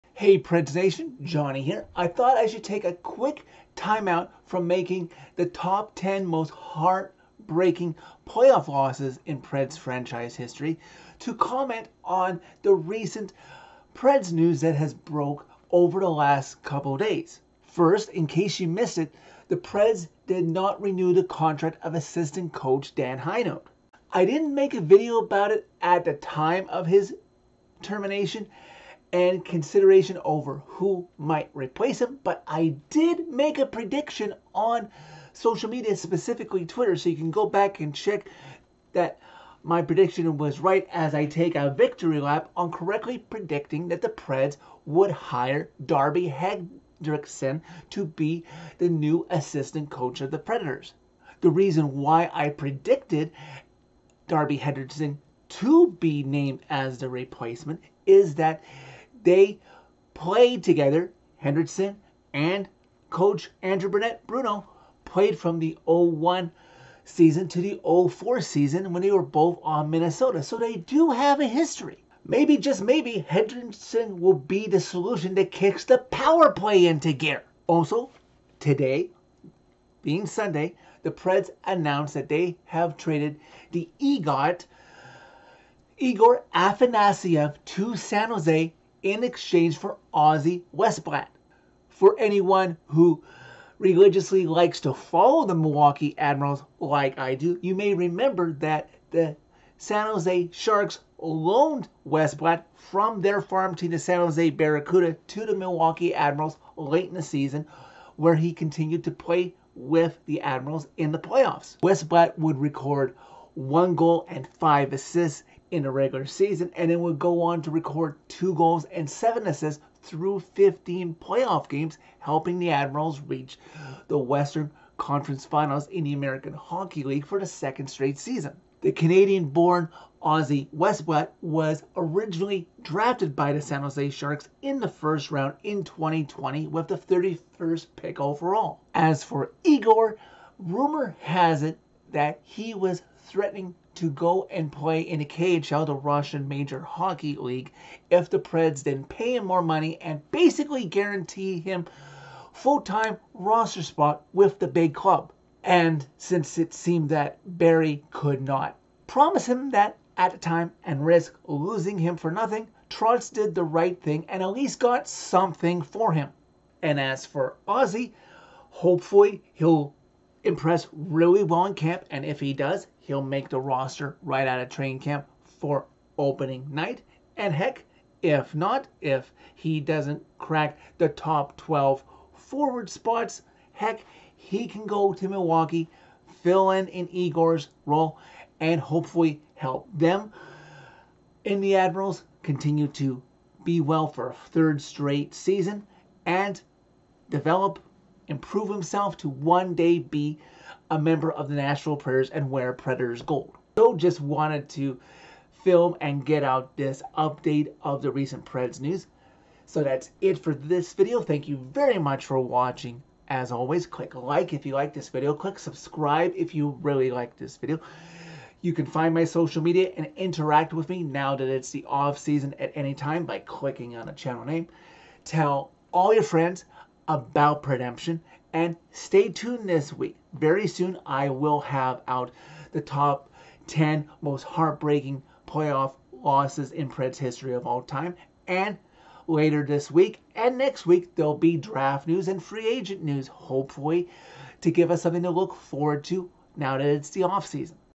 Coaching News & Egor for Ozzy
The latest Preds coaching news and the EGOAT is finding his way to San Jose!